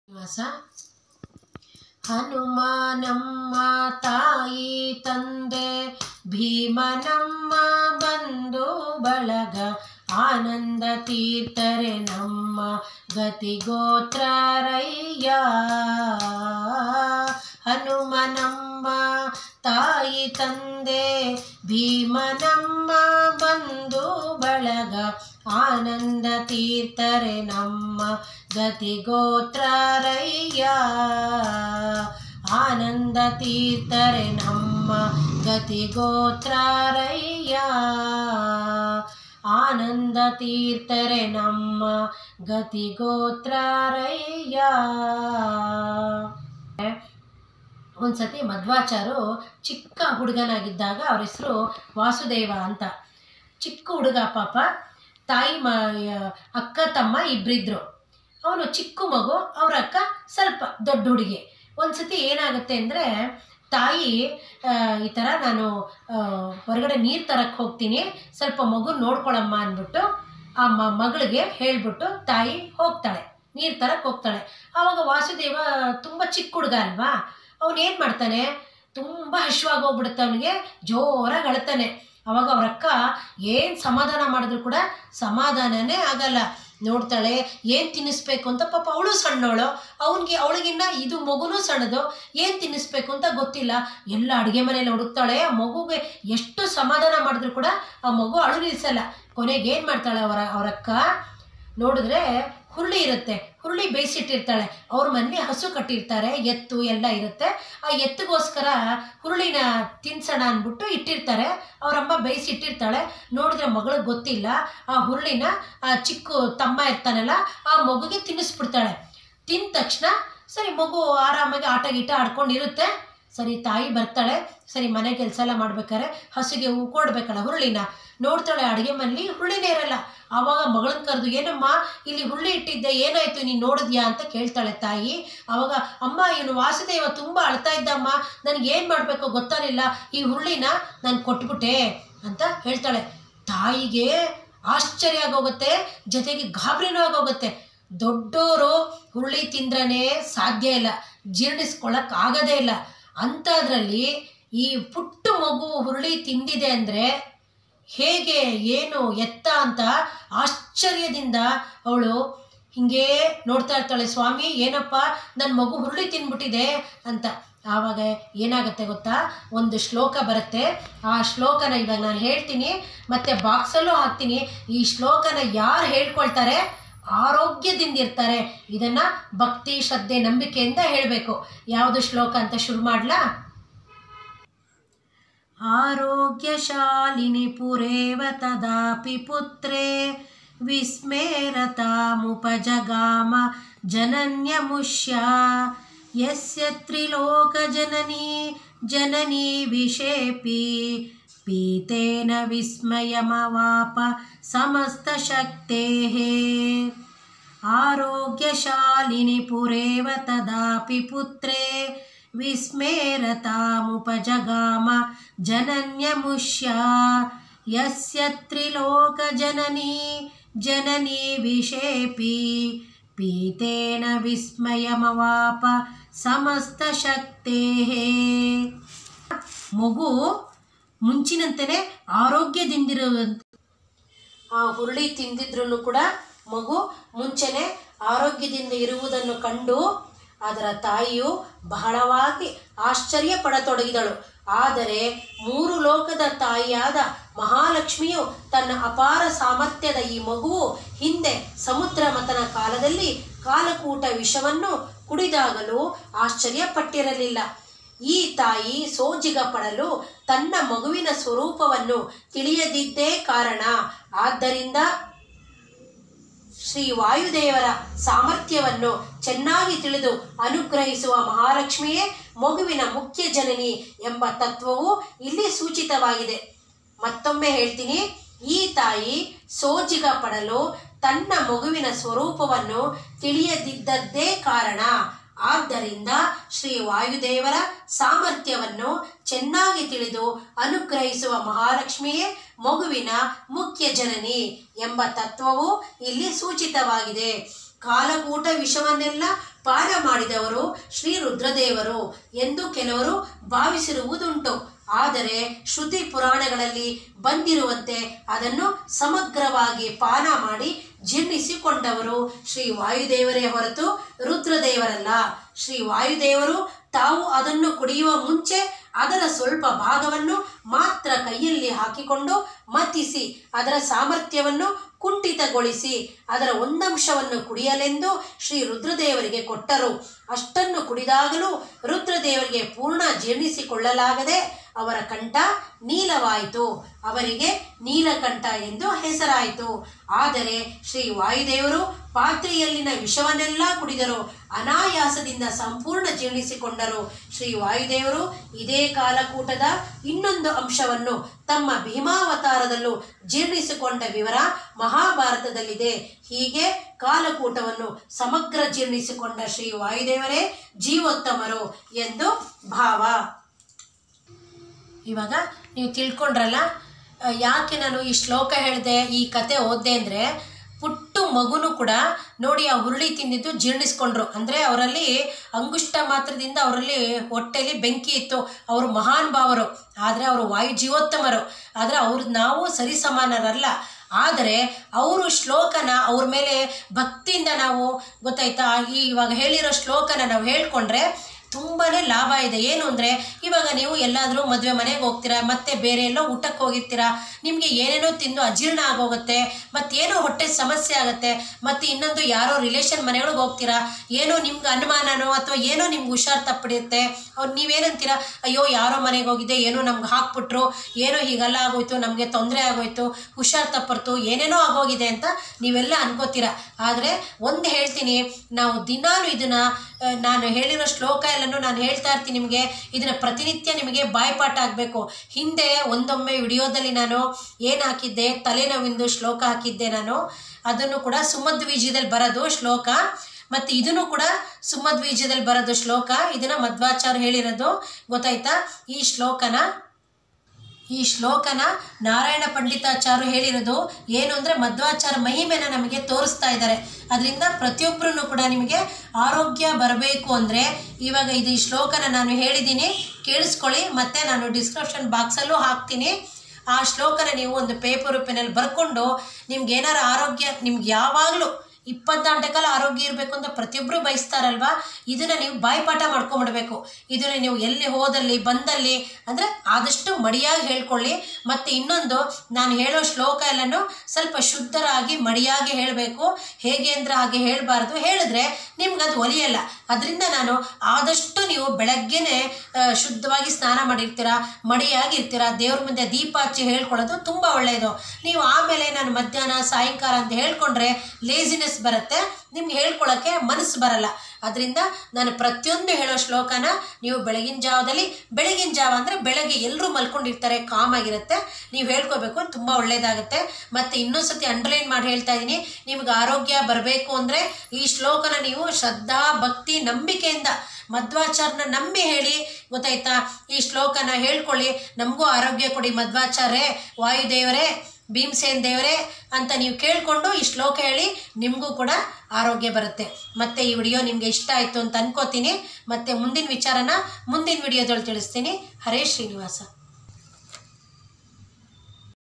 191 ಆರೋಗ್ಯ ಬೇಕೆ? ಈ ಶ್ಲೋಕ ಹೇಳಿ/Aarogya beke? E shloka heli
ಹರೇ ಶ್ರೀನಿವಾಸ, ನೀವು ಆರೋಗ್ಯವಂತರಾಗಿರಲು ಈ ಶ್ಲೋಕವನ್ನು ಶ್ರದ್ಧಾ ಭಕ್ತಿ ಮತ್ತು ನಂಬಿಕೆಯಿಂದ ತಪ್ಪದೇ ಹೇಳಬೇಕು

ಆರೋಗ್ಯಶಾಲಿನಿ ಪುರೇವ ತದಾಪಿ ಪುತ್ರೇ
 ವಿಸ್ಮೇ ರತಾಮುಪಜಗಾಮ ಜನನ್ಯಮುಷ್ಯ I
 ಯಸ್ಯ  ತ್ರಿಲೋಕ ಜನನೀ ಜನನೀ ವಿಷೇಪಿ
ಪೀತೇ  ನ ವಿಸ್ಮಯಮವಾಪ ಸಮಸ್ತ ಶಕ್ತೇ: II